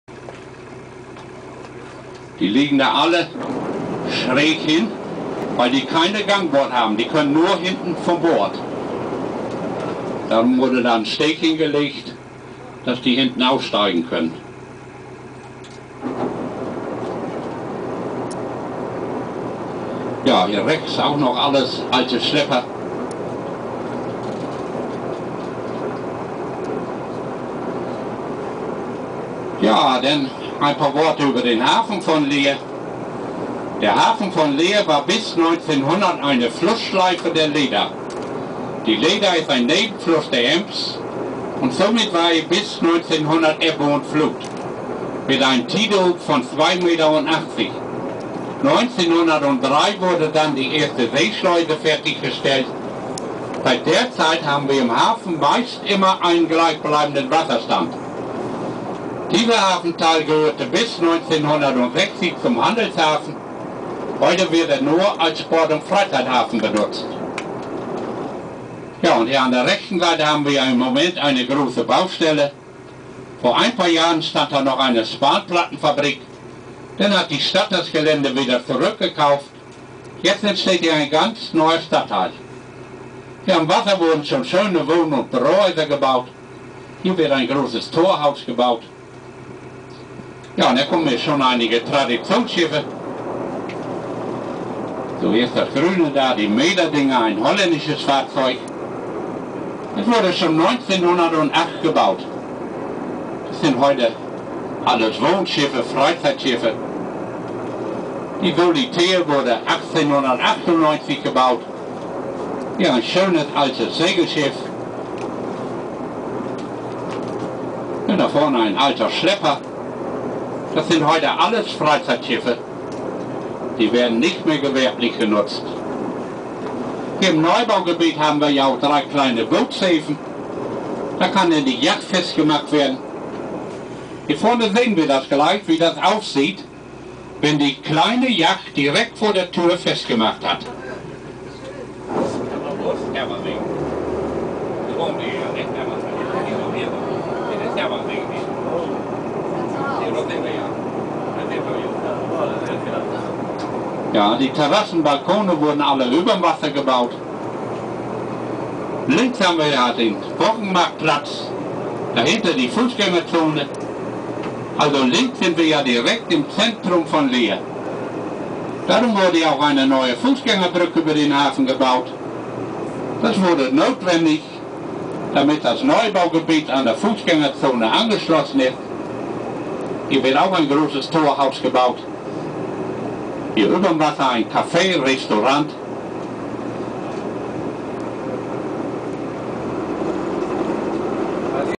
Hafenrundfahrt - Harbour sightseeing in Leer, Ostfriesland, Germany.
Visiting the city of Leer, we went for a sightseeing tour of the harbour.
August 2009.